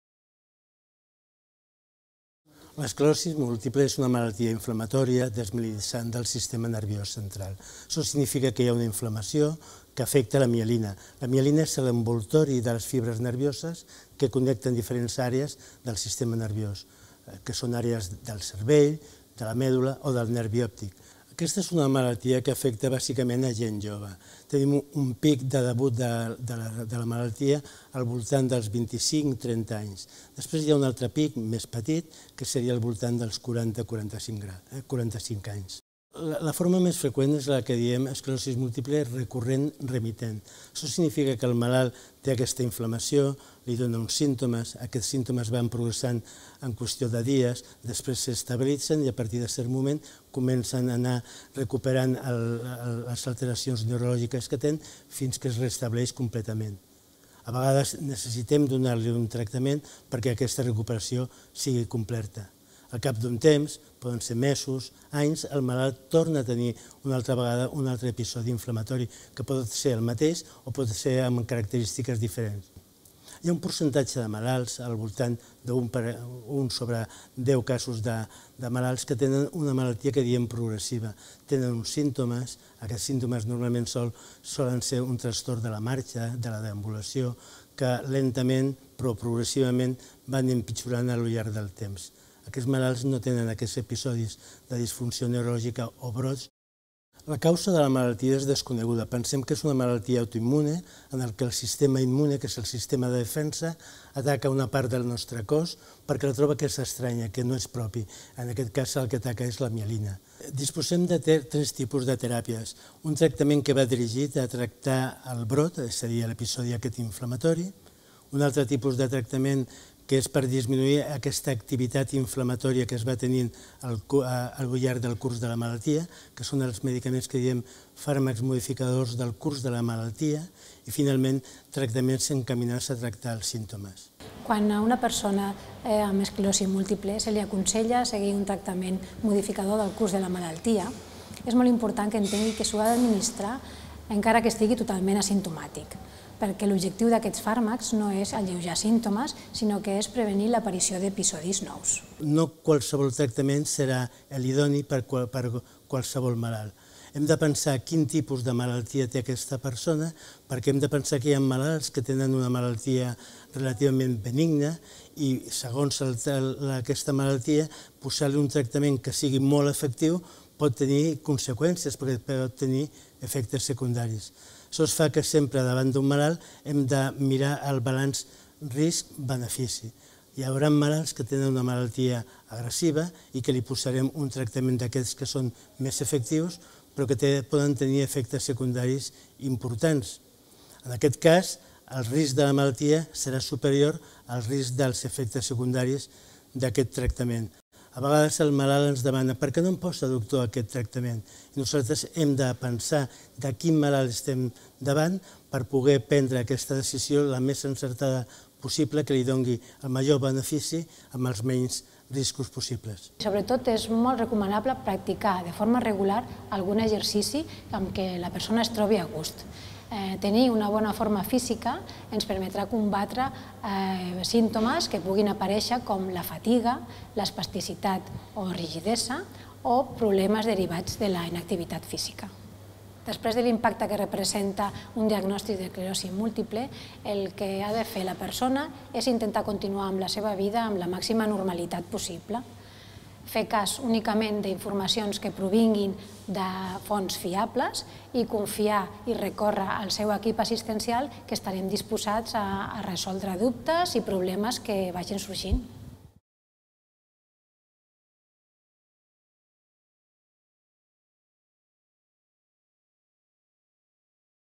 Esclerosi Múltiple | PortalCLÍNIC
L'esclerosi múltiple és una malaltia neurològica crònica que afecta el sistema nerviós central, és a dir, el cervell i la medul·la espinal. Els símptomes varien d'una persona a una altra i cada un evoluciona de diferent manera, en funció de l'àrea danyada. L'esclerosi múltiple és la segona causa de discapacitat neurològica en adults joves, després dels traumatismes, i afecta més a les dones que als homes.